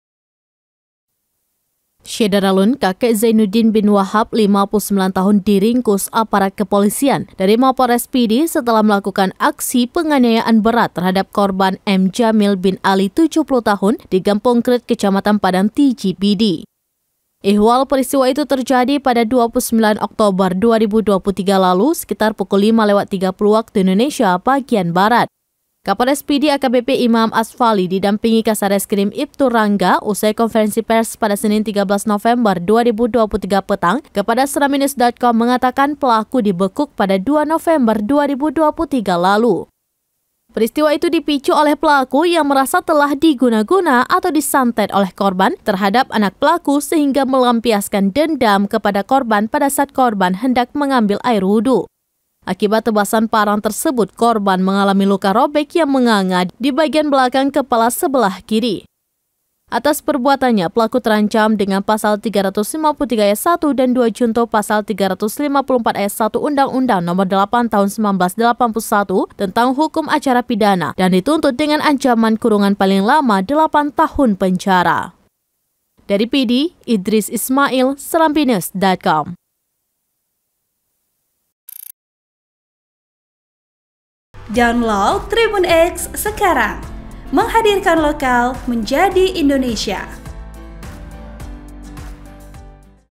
Kakek di Padang Tiji Tebas Tetangga Usai Merasa Diguna guna Korbannya
SERAMBINEWS.COM, SIGLI - Kakek Zainuddin bin Wahab (59) diringkus aparat Kepolisian dari Mapolres Pidie setelah melakukan aksi penganian berat terhadap korban M Jamil bin Ali (70) di Gampong Kreet, Kecamatan Padang Tiji, Pidie.

Ihwal peristiwa itu terjadi pada 29 Oktober 2023 lalu sekira pukul 05.30 WIB.

Kapolres Pidie, AKBP Imam Asfali SIK didampingi Kasat Reskrim Iptu Rangga Setyadi STrK MH usai konferensi pers, Senin (13/11/2023) petang kepada Serambinews.com, mengatakan, pelaku di bekuk pada 2 November 2023 lalu.

Laporan: Idris Ismail
Vo: Syita
Ev: Nash